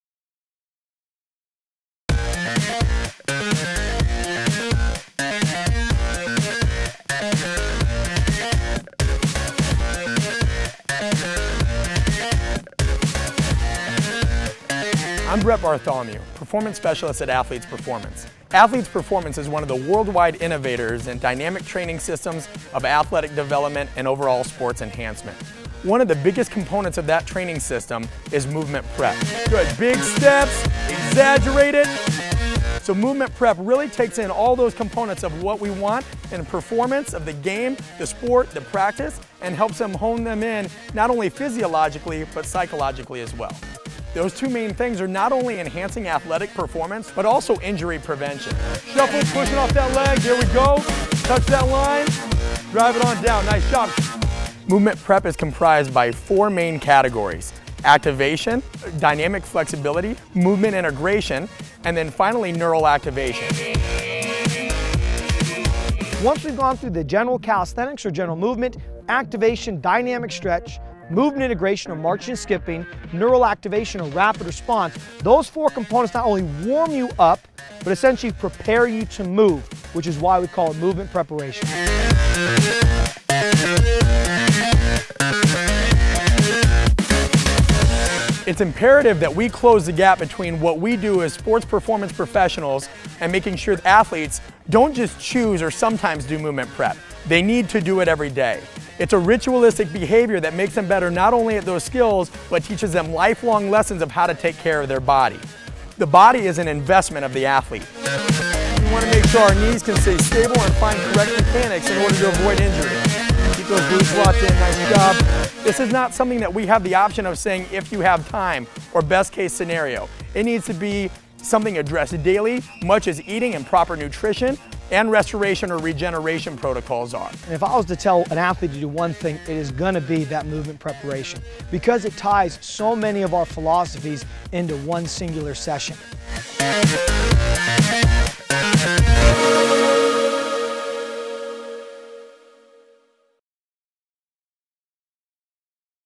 Movement Prep: AP & SKLZ
Prepare your body to perform the way it was meant to. Movement Prep features 4 main components geared to prepare your body for sport, the weight-room, and for speed & agility work.